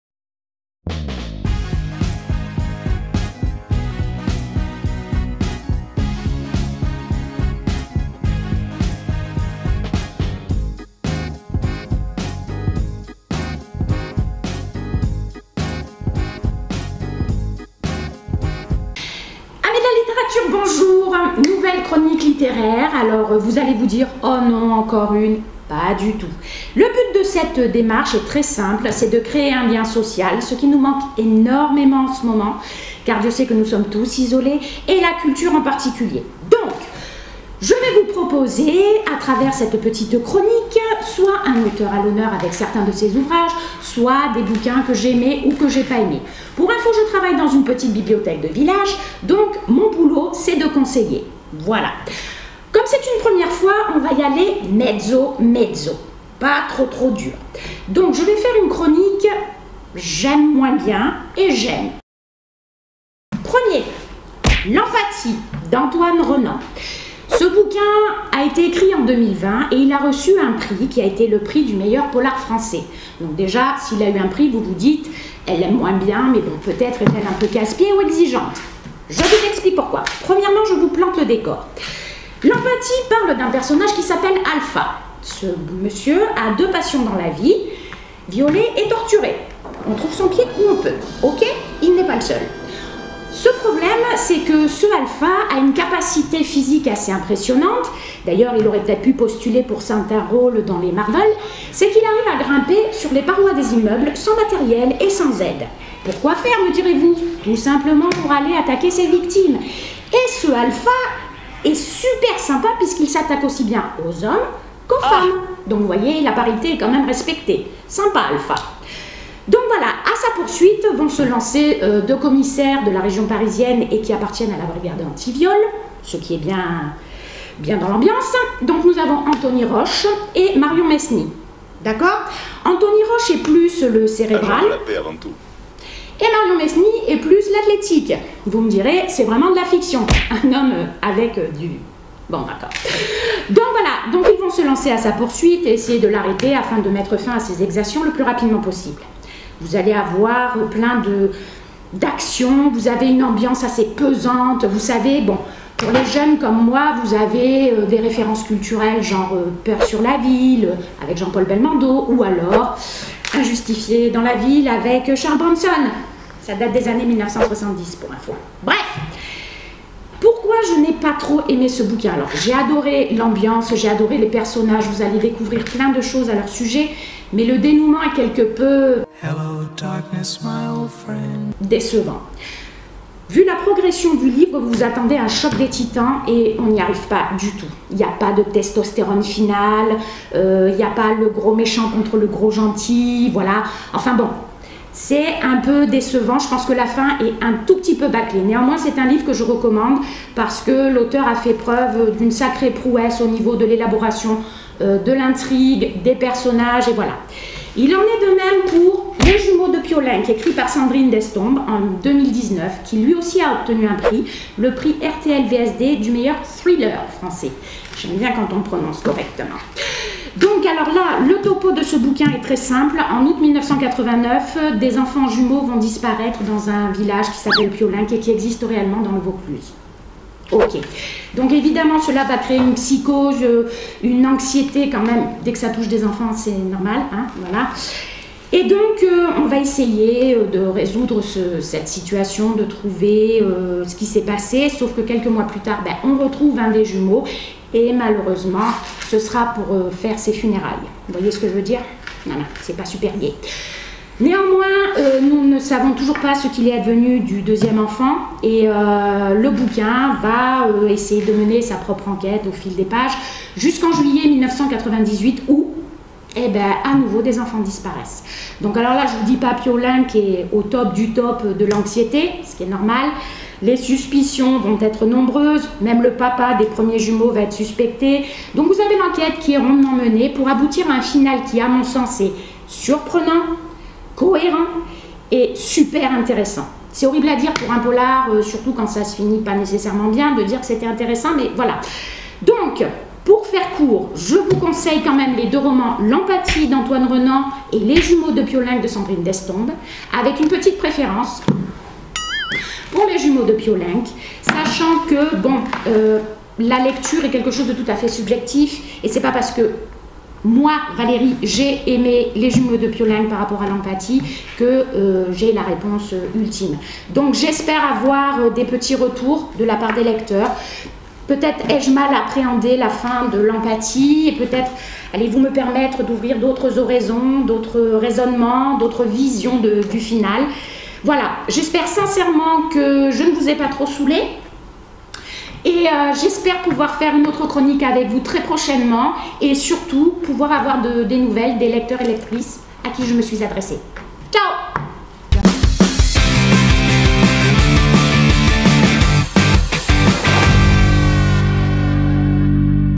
L'instant Bibli épisode 1 antoine renaud
Dans cette première vidéo de l'instant bibli nous allons échanger sur 2 romans :
- le premier l'Empathie d'Antoine RENAUD
- le second  Les jumeaux de Piolenc